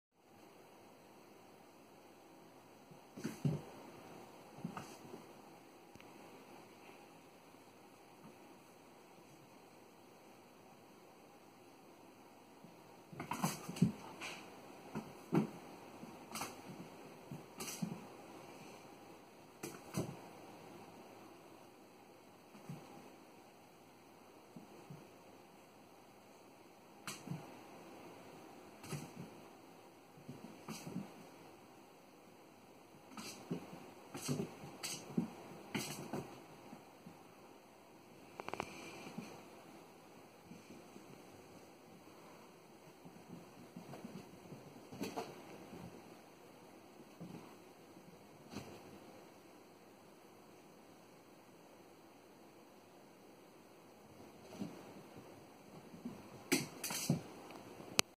Pinkie encounters the Red Dot
Foster kitten playing with laser pointer